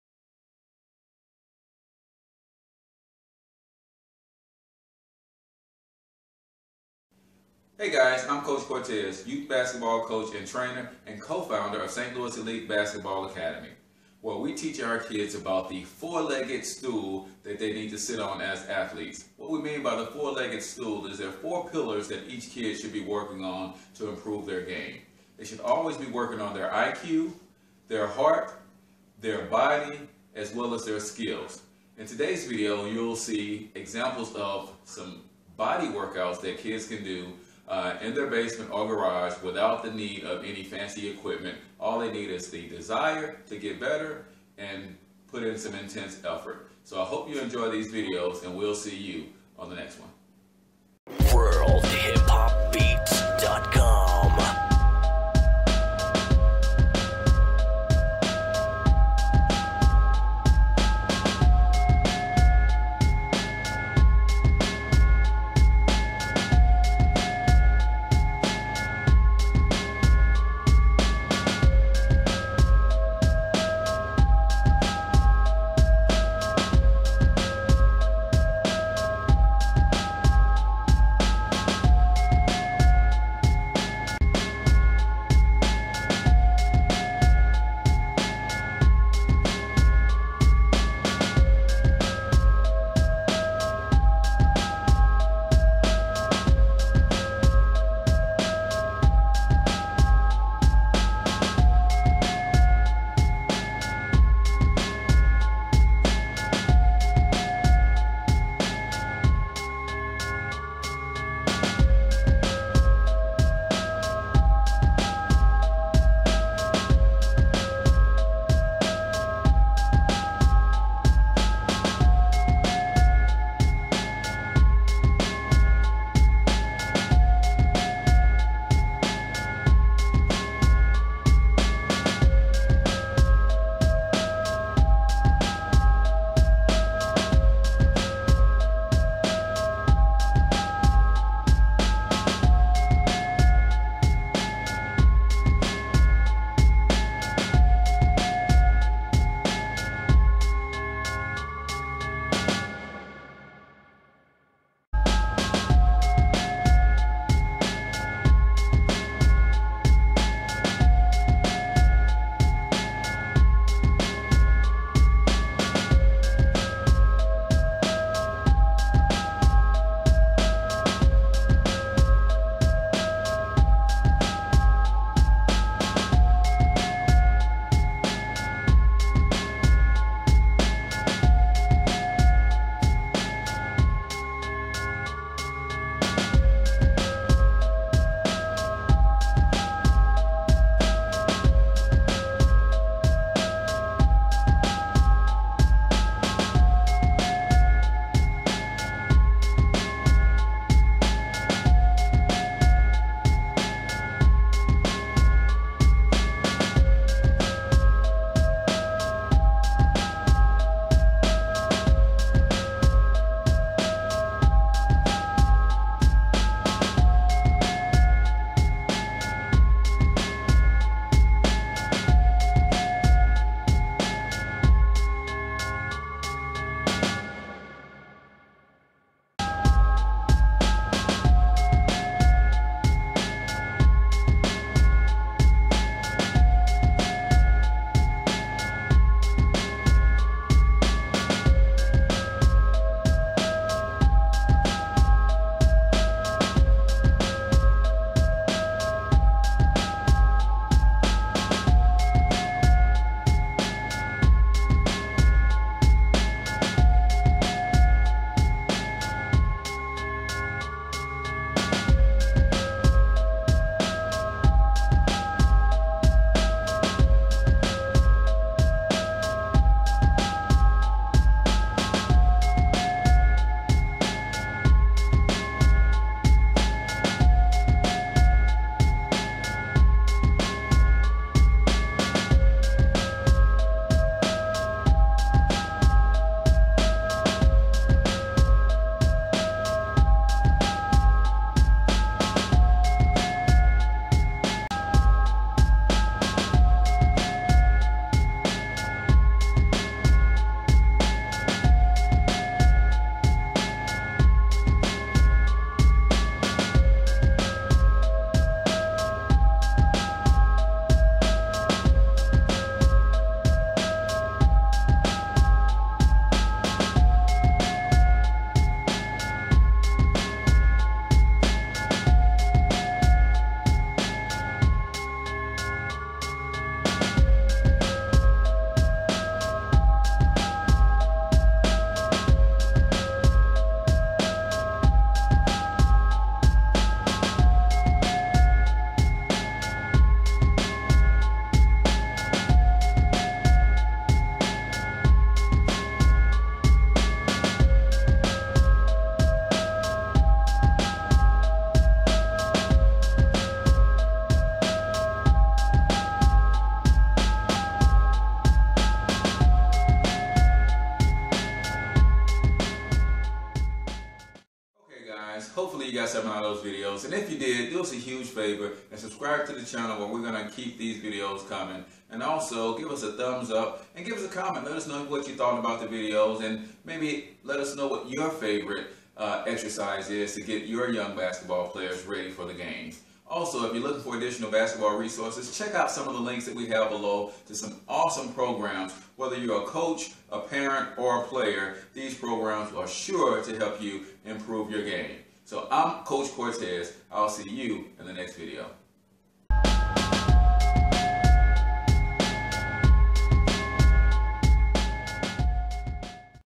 St Louis Elite Basketball Academy - Youth Workout
- Many youth athletes spend the majority of their time working to improve their skills but neglect their body. This video will hopefully inspire young players to start performing some of these simple body weight exercises to improve their physical strength, endurance agility and jumping ability. These exercises can be done by any aspiring athlete no matter the age.